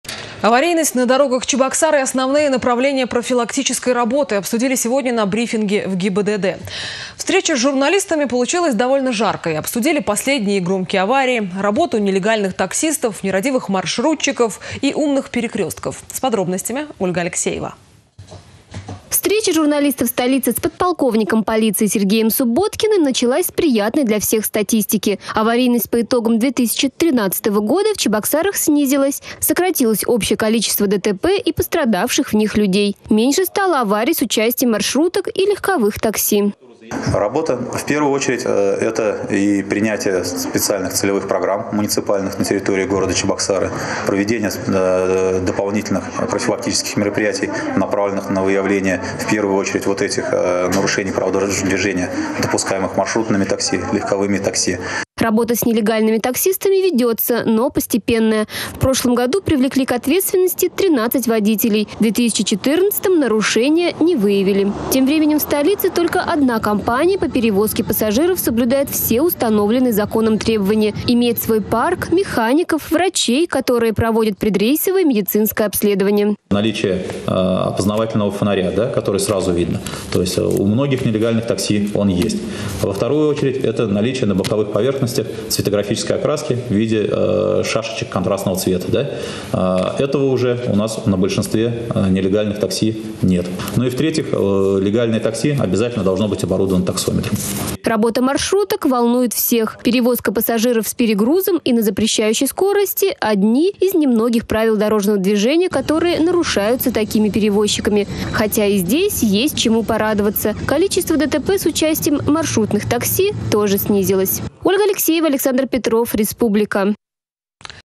Аварийность по итогам 2013 года в Чебоксарах снизилась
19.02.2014
Аварийность на дорогах Чебоксар, меры предупреждения и основные профилактические работы Госавтоинспекции обсудили сегодня на брифинге в ГИБДД. Встреча с журналистами получилась довольно жаркой: обсудили последние «громкие» аварии, работу нелегальных таксистов, нерадивых маршрутчиков и «умных» перекрестков.